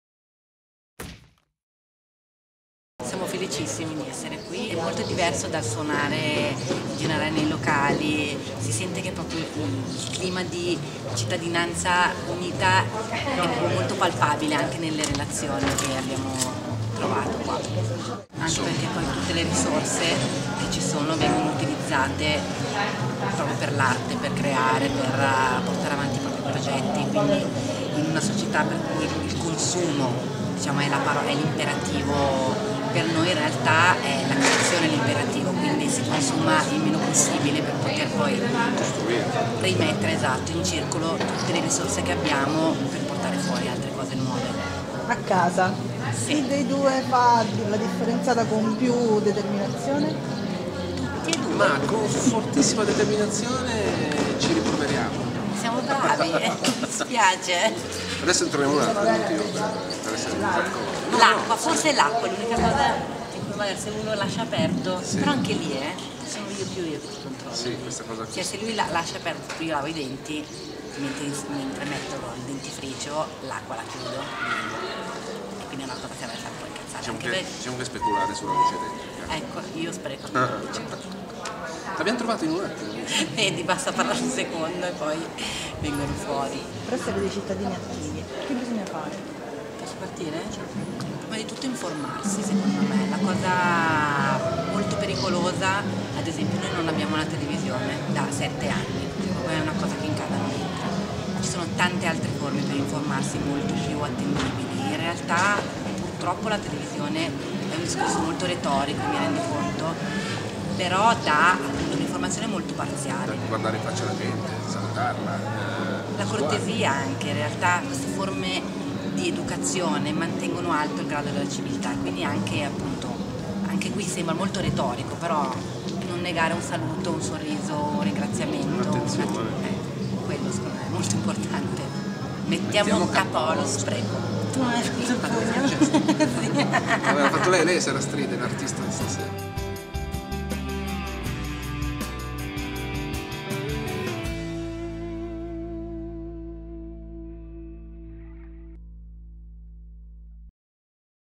Intervista a Sarah Stride
Intervista ai protagonisti di Sprek.o. 2015 , la  Festa nazionale per la lotta agli sprechi.
Tre giorni di laboratori, workshop ed eventi per imparare a riconoscere i comportamenti sbagliati e combattere insieme contro gli sprechi.